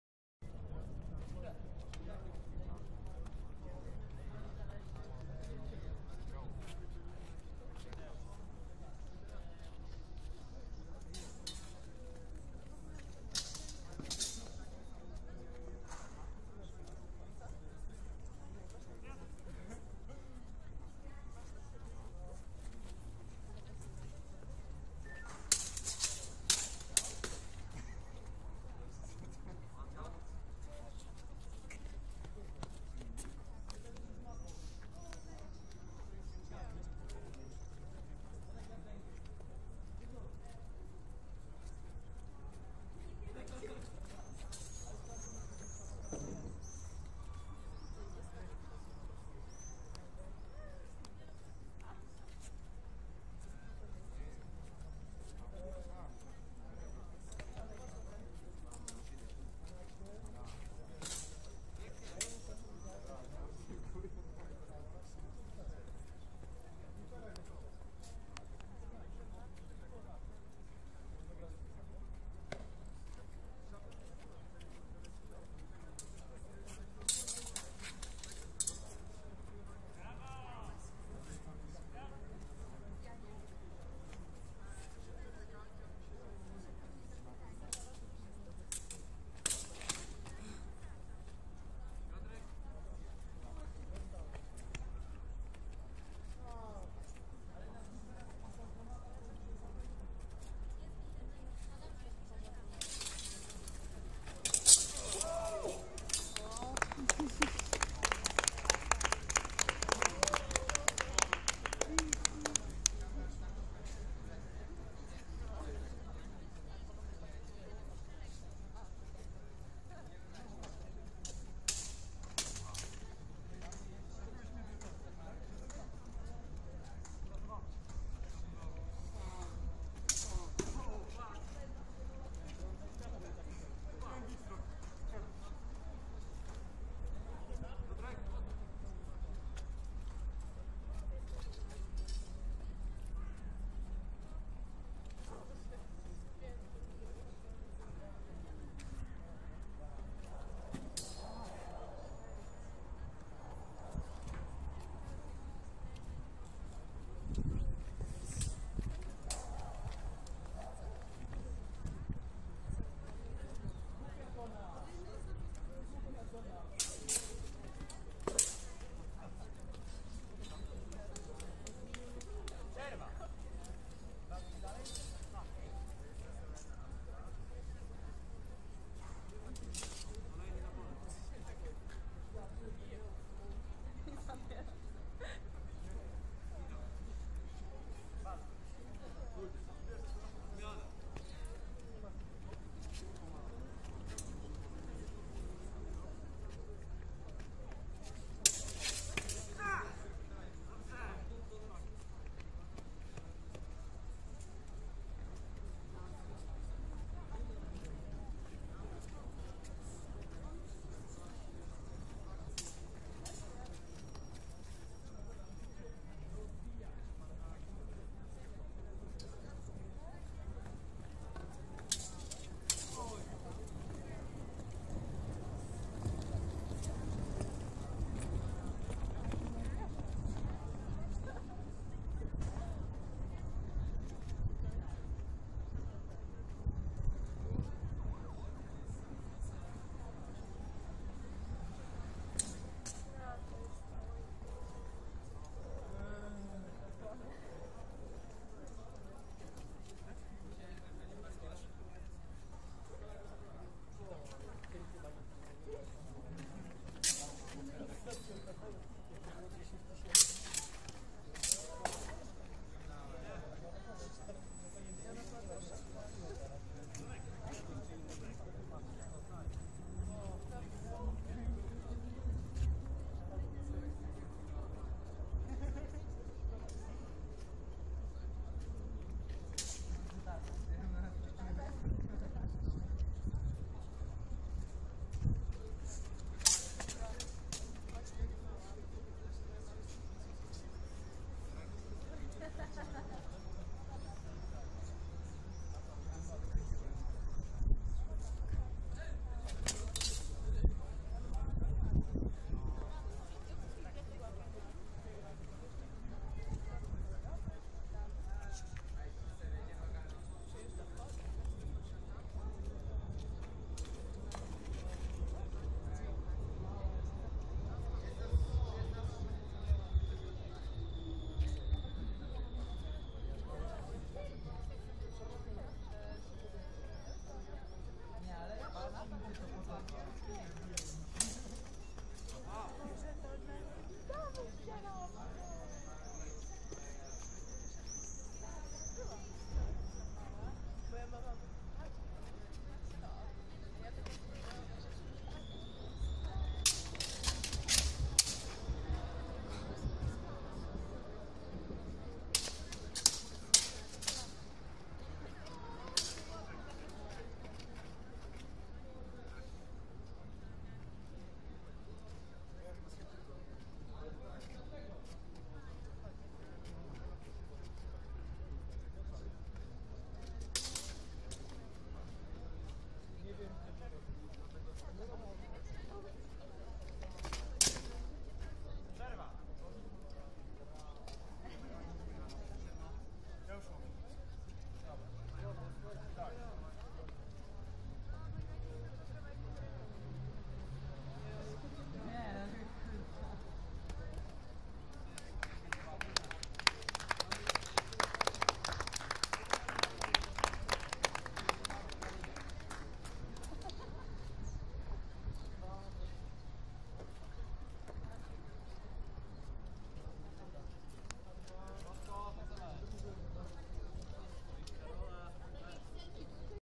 Piotr Paprzycki vs Wszyscy AVANGARDA 2014
Seria pojedynków przy okazji Konwentu Avangarda 2014